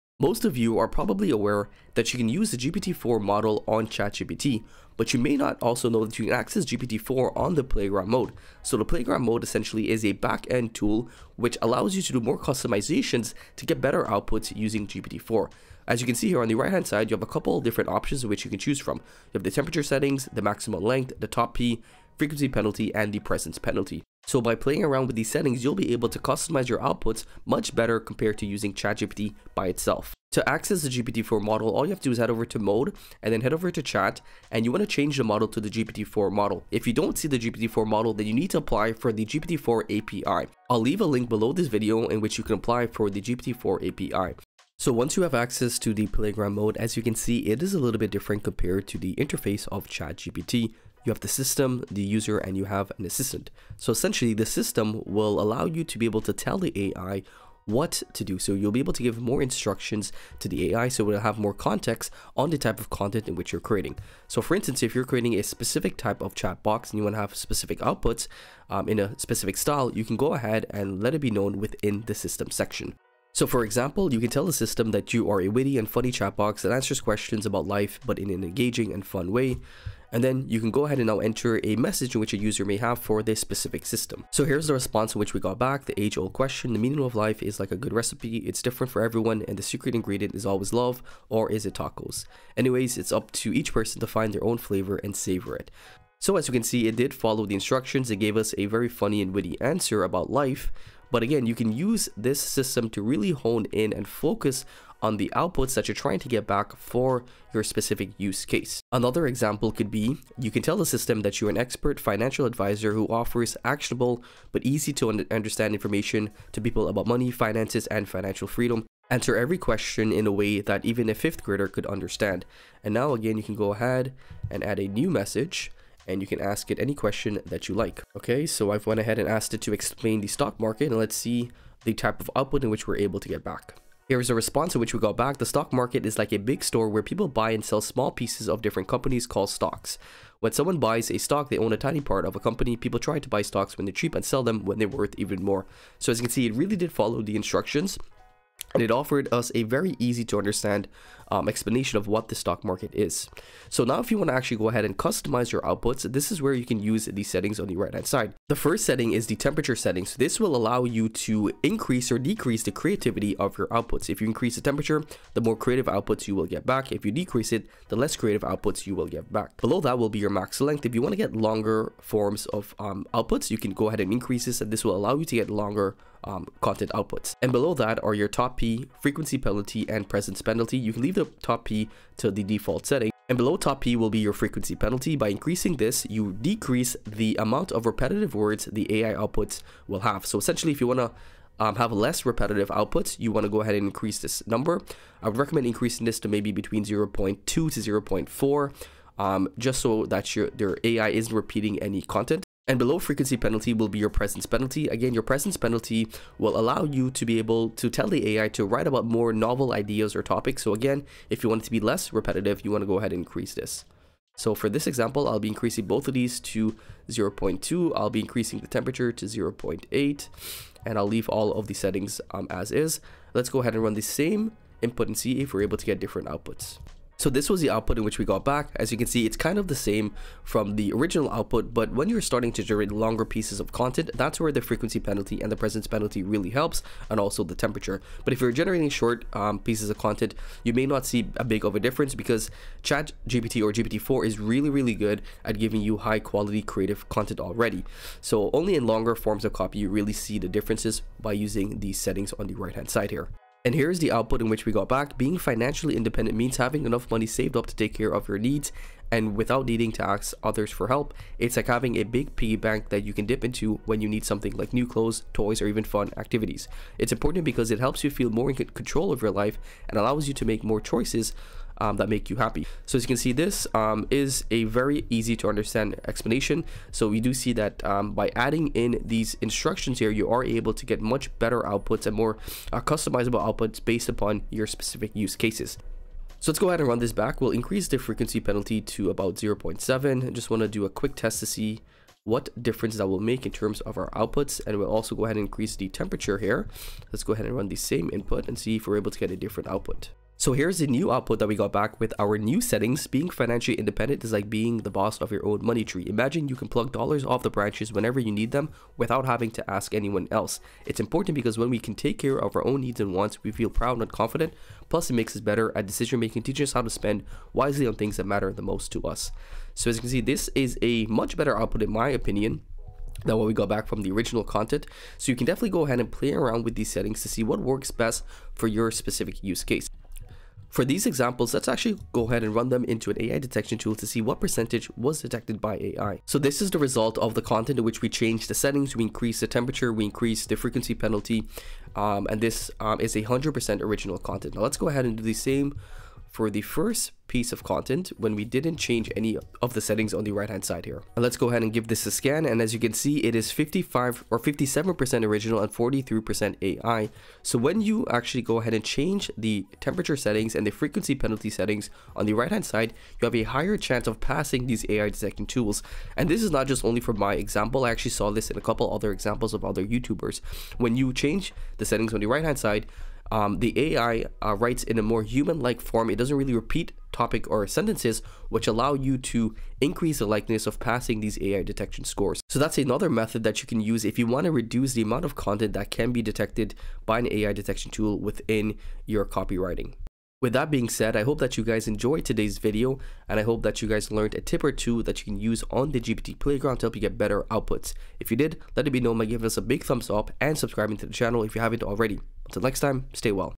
How To Supercharge GPT-4 For 10x Output Quality (Must Watch)
In todays video i show you how to supercharge gpt 4 using the plaground mode.

-how to access gpt 4 in the playground mode
-how to apply for gpt 4api
-how to use the gpt 4 playground mode
-how to generate higher quality content using the gpt 4 playground
-how to generate better content that is less likely to be detected by ai using the playground mode
-how to use got 4 and get better outputs